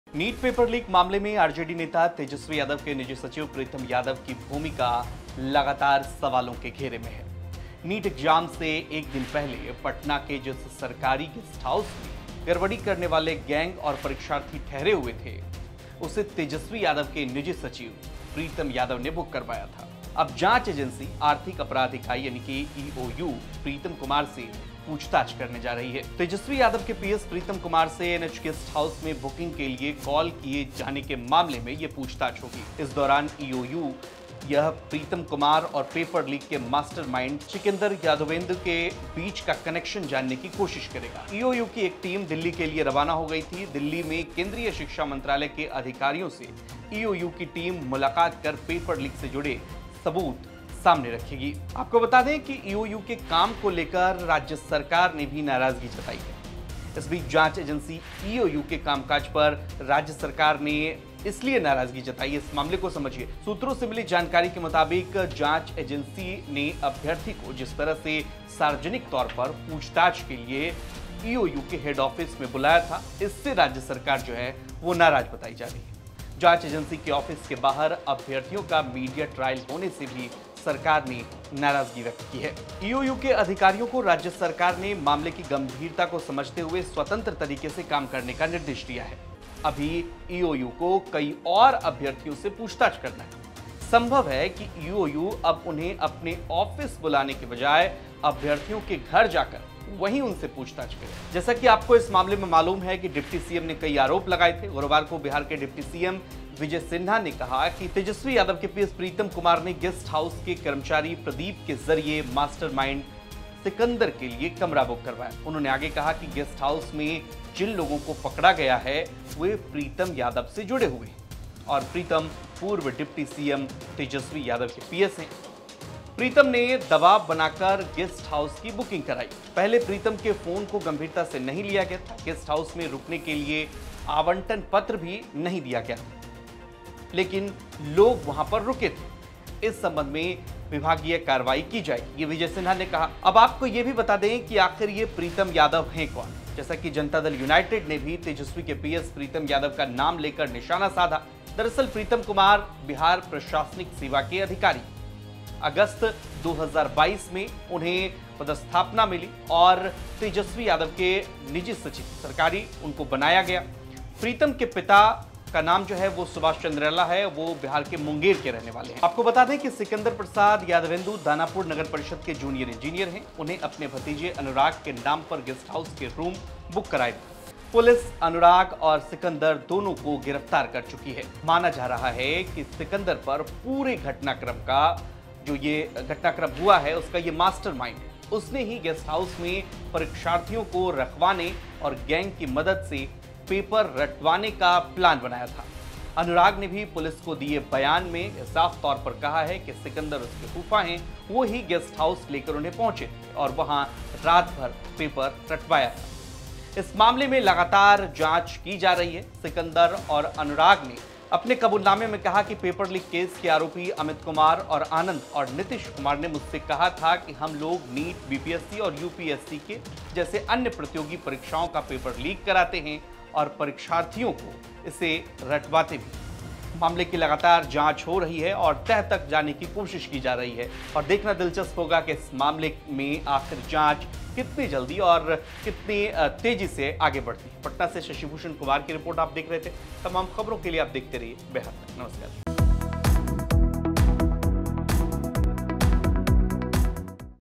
NEET Paper Leak: Tejashwi के PS से पूछताछ करेगी EOU, जांच एजेंसी के काम से राज्य सरकार नाराज
तेजस्वी यादव के पीएस प्रीतम कुमार से NH गेस्ट हाउस में बुकिंग के लिए कॉल किए जाने के मामले में यह पूछताछ होगी.